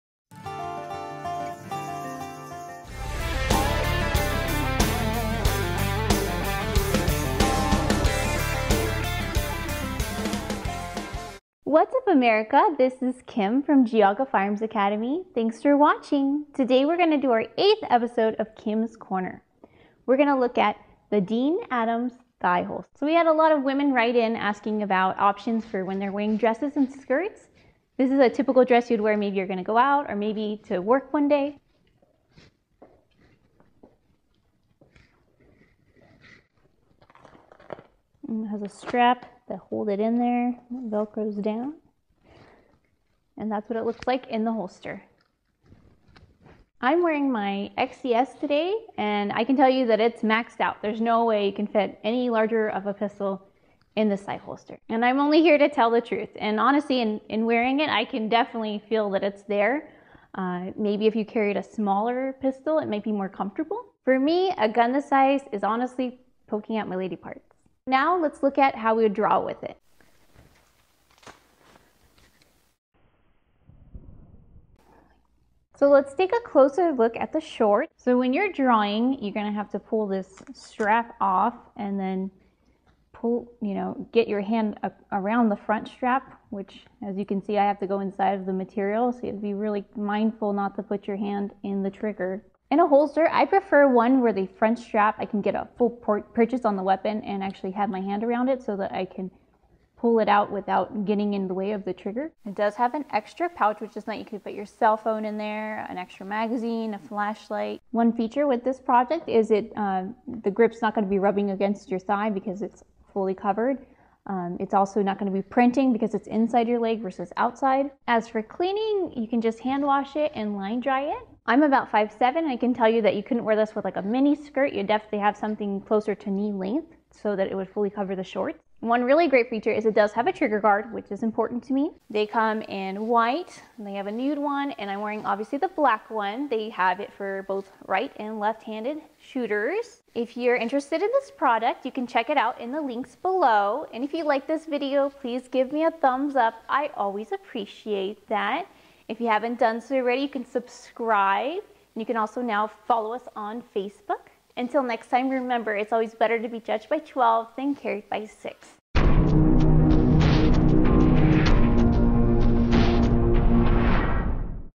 Female Thigh Holster Review: Dene Adams Thigh Holster Shorts | Geauga Firearms Academy
The Dene Adams Body Shaping Thigh Holster Shorts securely holsters and conceals your gun in a sub-layer of the shorts body shaping compression. The thigh holster is made of a soft moderate support Lycra. A high waist smooths the tummy and a clean finish around the base of your thigh prevents rolling or riding up of the short.

Thigh holster features:
1) Fast breakaway retention tab holsters your gun securely in place
2) Thigh holster great for all seasons, all day/night wear
3) No gun grips rubbing your thighs
4) No printing through your skirt